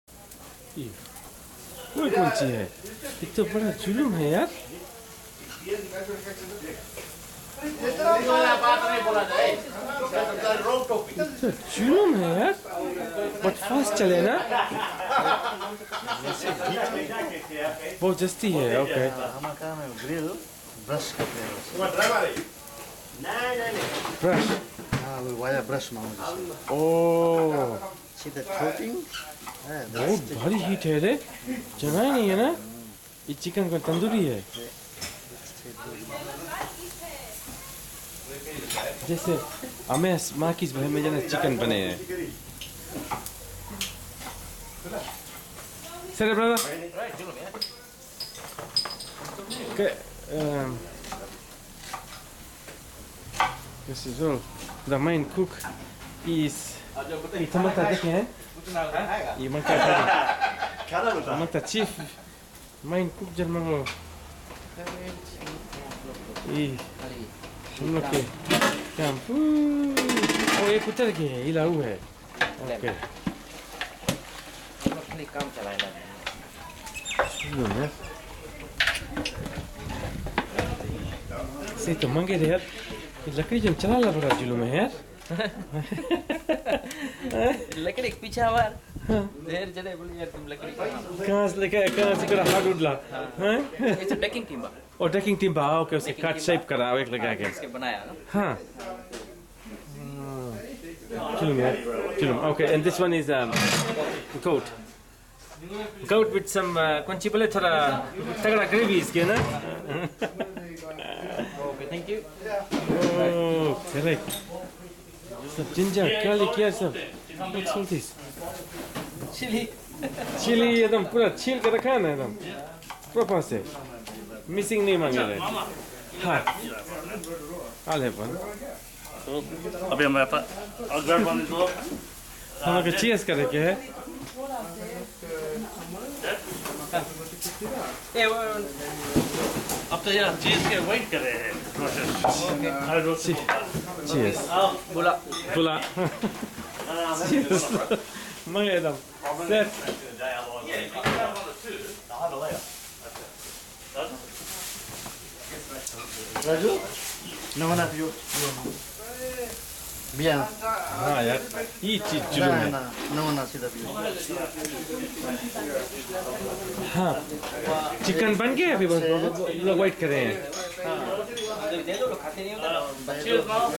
Fiji Indian Tandoori BBQ|Goat and Chicken Curry
This food preparation was in western Sydney, a family residence all are Fiji Indians. The initial scenes are about Tandoori chicken BBQ - chicken pieces marinated overnight in home made tandoori paste and cooked over hot coal. It depends on how big chicken pieces you have, depends how long you marinate for. You should be able to control the heat coming from the coal as it can easily overcook however you want the burnt slightly to give it some taste from the village, like from the olden days!
The chef has prepared chicken curry pieces (with bones) and goat curry using traditional fiji indian spices - premium curry mix from island stores within Australia and New Zealand. Lot of ginger, inions, garlic, tomato, curry leaves, coriander and spices are used. The neighbours loved the food!